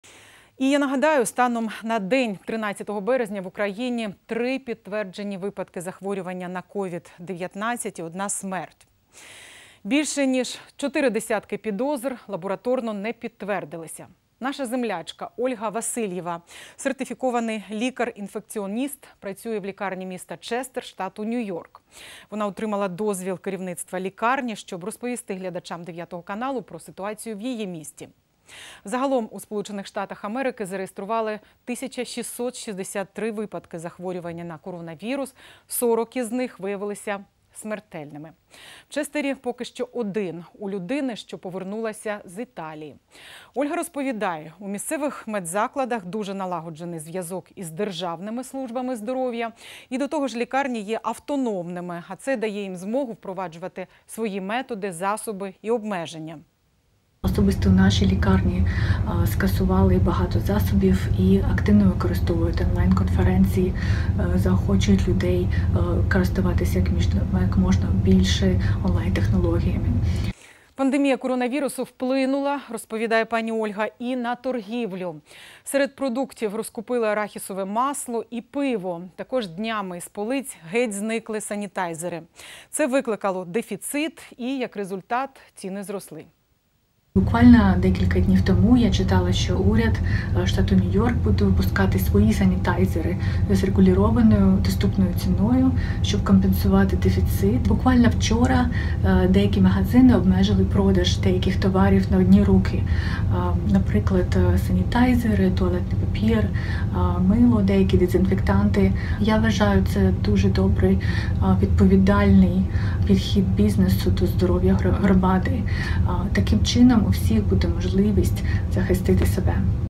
Українська лікарка, що працює в США, розповіла 9 каналу про ситуацію в Рочестері
Наша землячка Ольга Васильєва, сертифікований лікар-інфекціоніст,  працює в лікарні міста Рочестер штату Нью-Йорк. Вона отримала дозвіл керівництва лікарні, щоб розповісти глядачам 9 каналу про ситуацію в її місті. Загалом у Сполучених Штатах Америки зареєстрували 1663 випадки захворювання  на коронавірус, 40 із них виявилися смертельними. В Рочестері  поки що один - у людини, що повернулася з Італії. Ольга розповідає: в місцевих медзакладах дуже налагоджений зв'язок із державними службами здоров'я.  До того ж лікарні є автономними, а це дає їм змогу впроваджувати свої методи, засоби і обмеження. Пандемія коронавірусу вплинула, розповідає пані Ольга, і на торгівлю. Серед продуктів розкупили арахісове масло і пиво, також  днями з полиць геть  зникли санітайзери. Це викликало дефіцит і, як результат ціни зросли.